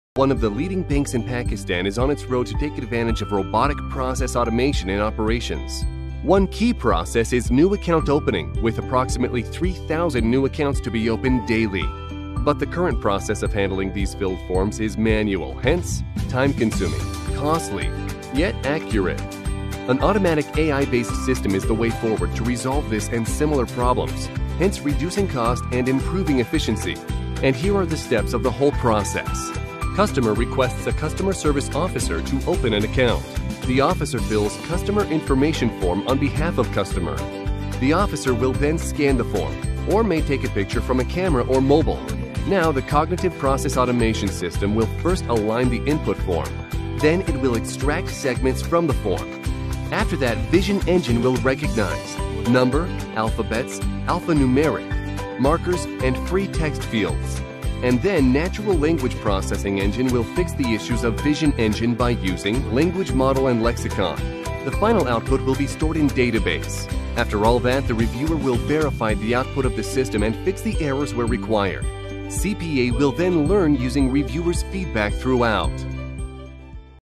OCR in Banking || RPA
Discover the power of Optical Character Recognition (OCR) technology in the banking industry. Our cutting-edge OCR software streamlines banking processes by accurately scanning and converting physical documents into digital format. With OCR in banking, manual data entry becomes a thing of the past and banks can improve efficiency, reduce errors, and save time and resources. Experience the benefits of digital banking with OCR technology and transform the way you manage your operations.